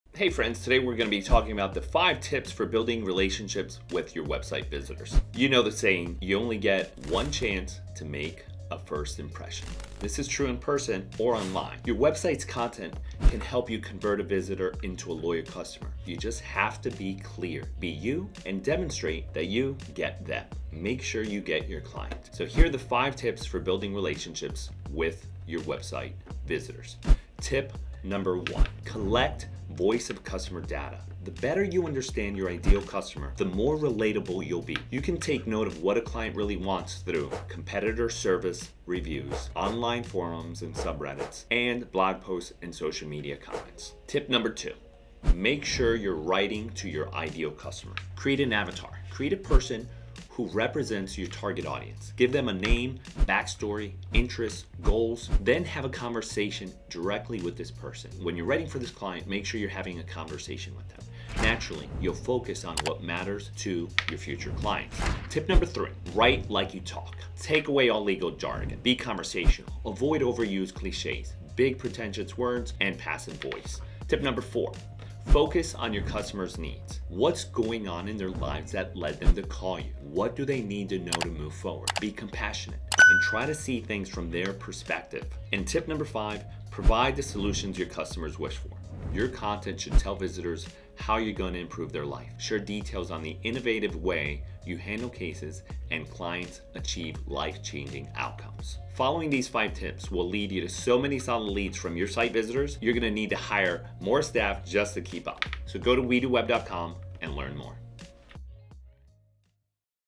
5 Tips on Building Relationship with Your Law Firm Website Visitors
Want to build a stronger connection with your law firm website visitors? Follow these five simple tips to build a stronger connection with your audience and keep them coming back for more.